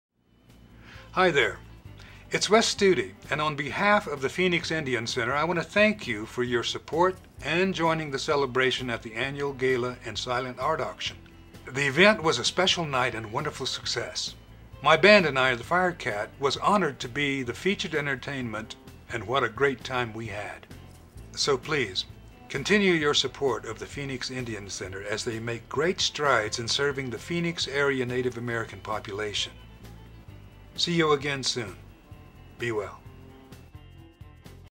THANK YOU from Wes Studi and PIC
Thank you for celebrating 60 years of the Phoenix Indian Center with us!

The Phoenix Indian Center is the oldest urban-based nonprofit organization serving the needs of American Indians. For more: